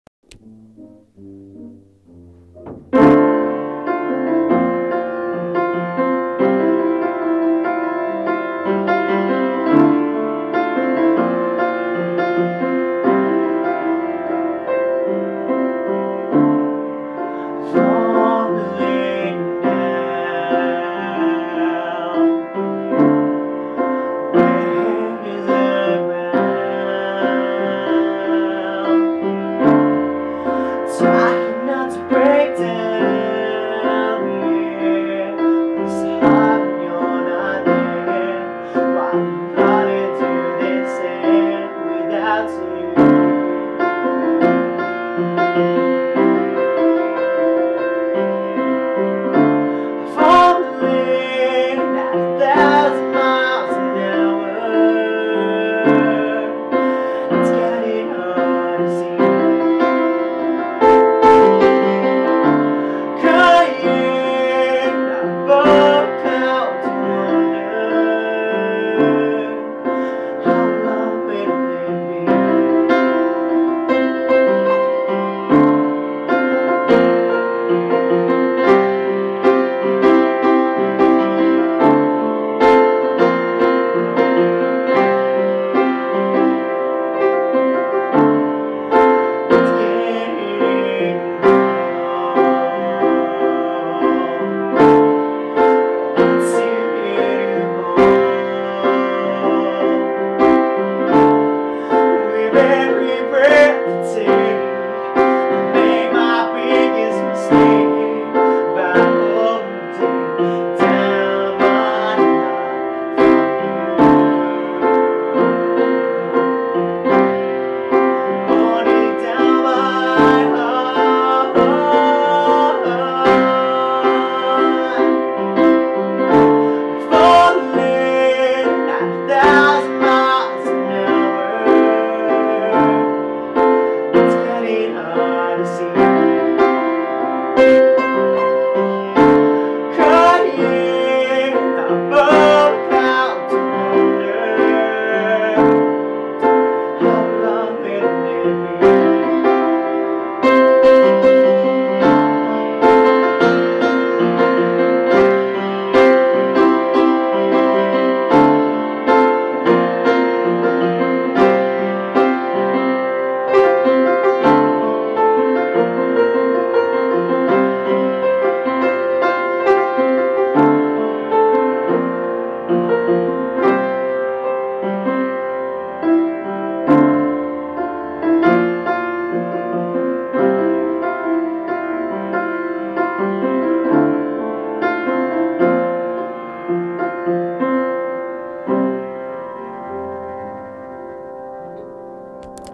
A composition
A song i composed recently. Just thought.. i'd share it around ;)

Lyrics:

Falling down, wheres the ground?
Trying not to break down
But its so hard when your not there, while im falling through this air without you.

Falling at a thousand miles per hour,
its getting hard to see.
Crying above the clouds i wonder,
how long will it be?

Its getting cold, without you here to hold.
With every breath i take, i make my biggest mistake.
By holding down my heart from you.

Holding down my heart

Cryin above the clouds i wonder,